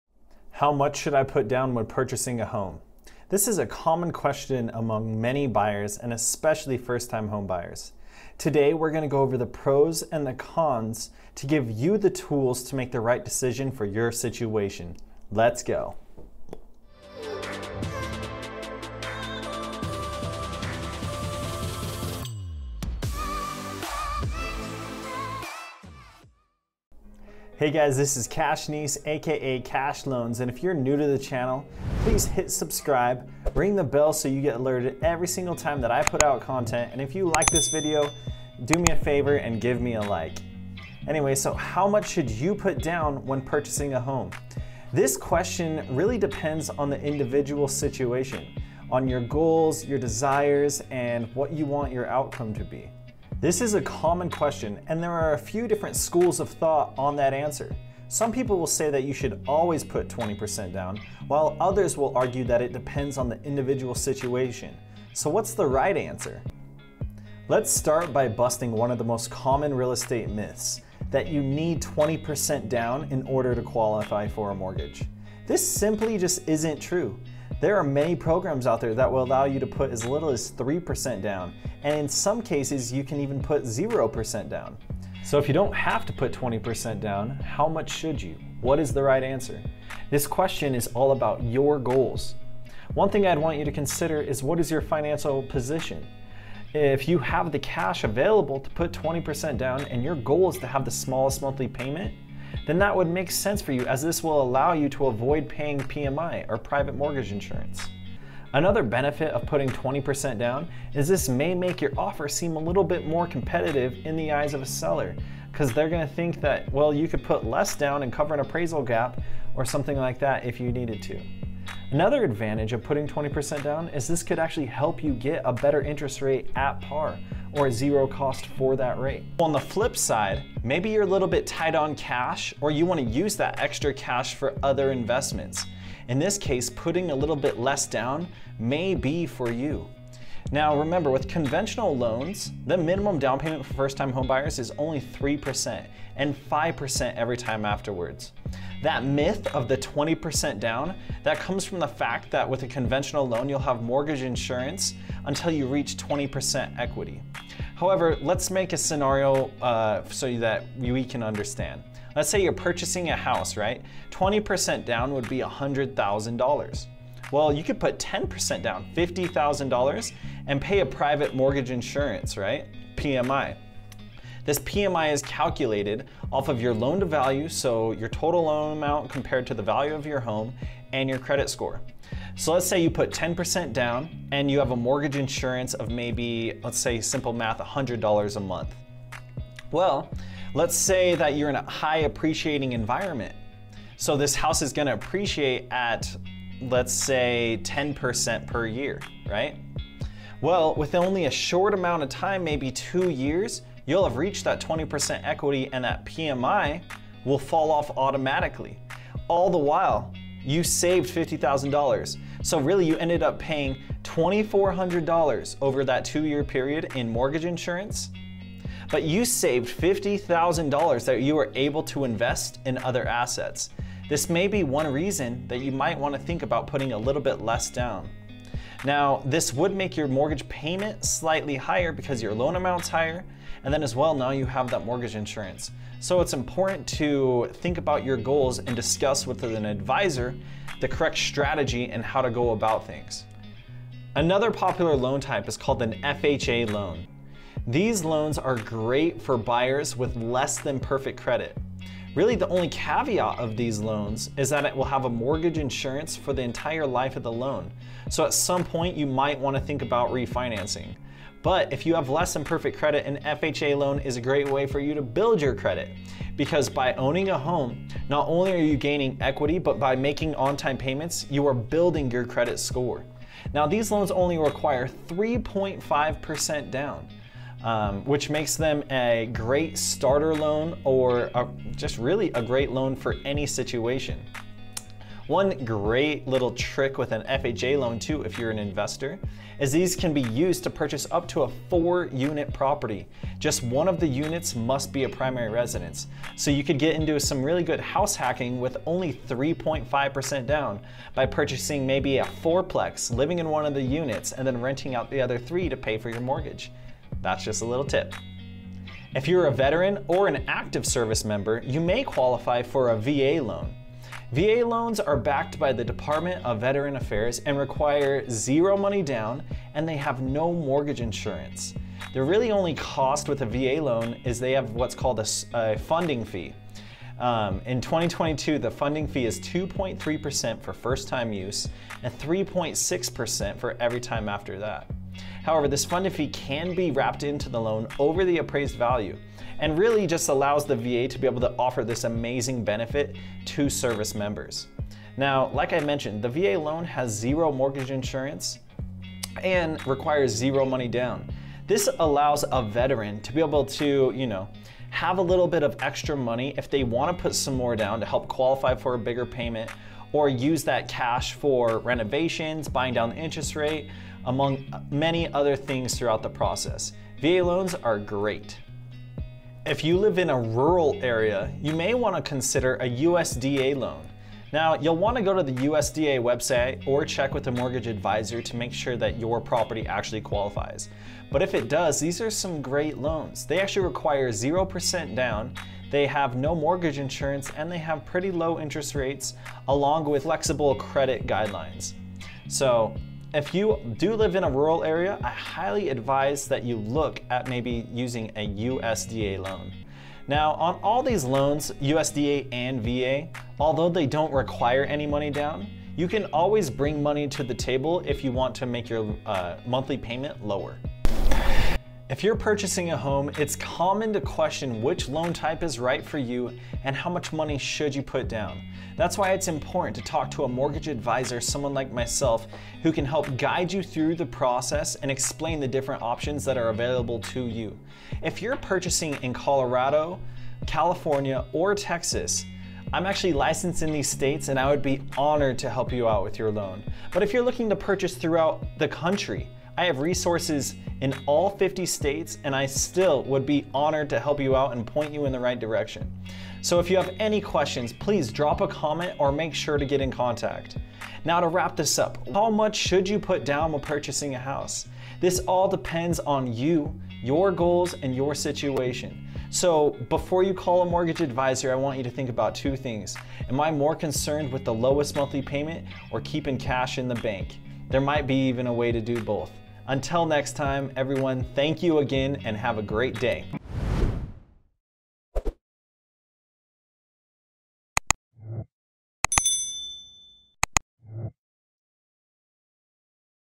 How Much Should I Put Down When Purchasing A Home?
How much should I put down when purchasing a home? When you're purchasing a home, there are a lot of things to consider. One of the biggest is how much money you should put down as a down payment.

There are a few different schools of thought on this. Some people believe that you should put down as much as possible, so that you have more equity in your home from the start. Others believe that you should put down the minimum amount required by your mortgage lender, so that you can keep more of your money in savings.

The truth is, there is no right or wrong answer. It really depends on your individual circumstances and what your goals are.

If you have a lot of money saved up and you're looking to build equity quickly, then putting down a larger down payment is a good idea. On the other hand, if you're tight on cash and you need to keep as much money in savings as possible, then a smaller down payment may be the way to go.

You should also be aware that there are different types of mortgages available, each with their own requirements. For example, some loans require a minimum down payment of 3%, and some even offer financing with 0% down.

The best way to figure out how much you should put down on your home is to speak with a licensed mortgageadvisor to help guide you through the process, and lay out all the options so you can make the right decision for YOU!

I hope this video has helped answer the question of how much you should put down when purchasing your first home.

Cache Nies
Mortgage Advisor
NMLS # 1793984

00:00 Introduction
01:06 Busting the 20% down payment myth
01:25 When putting 20% down makes sense - Conventional
02:09 When it makes sense to put less than 20% down -  Conventional
04:00 FHA Loans - 3.5% Down
05:08 VA Loans - 0% Down
06:08 USDA Loans - 0% Down
06:52 Speak with an advisor to go over your goals, and make the right decsion for YOU!